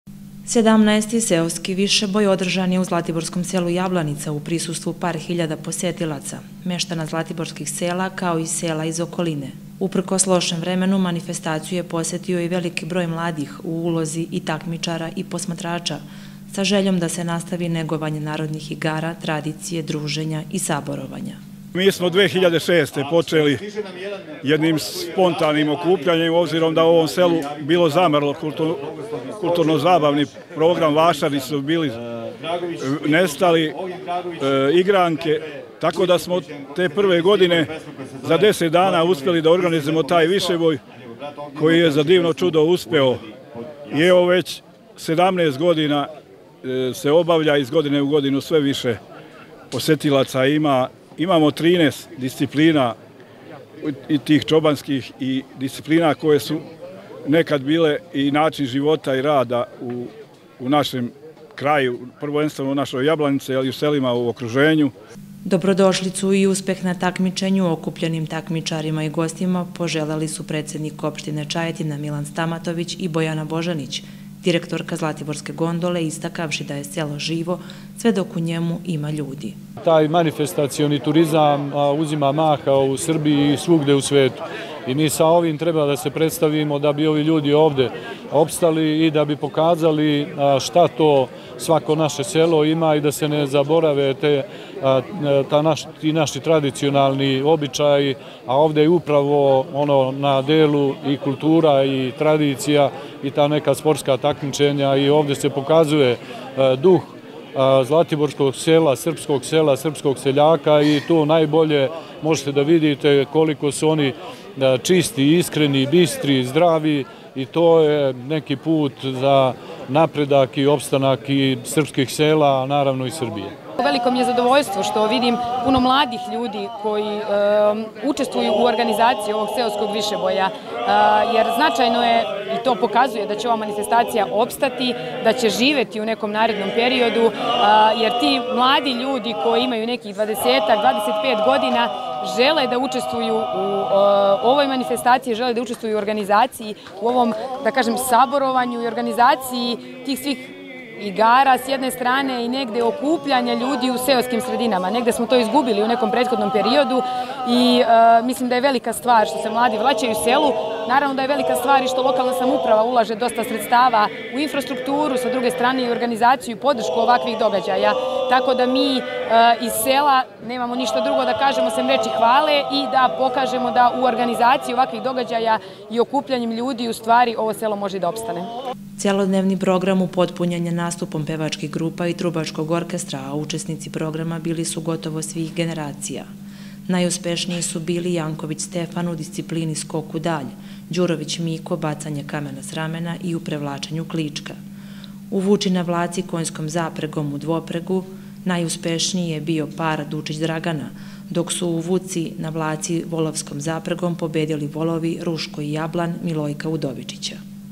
Viseboj 2023
У златиборском селу Јабланица и ове године одржана је надалеко позната манифестација – Сеоски вишебој. Организатор традиционалног Вишебоја је ТУ Јабланица у сарадњи са месном заједницом овог села, као и јавним установама и предузећима општине Чајетина, а покровитељ је Општина Чајетина.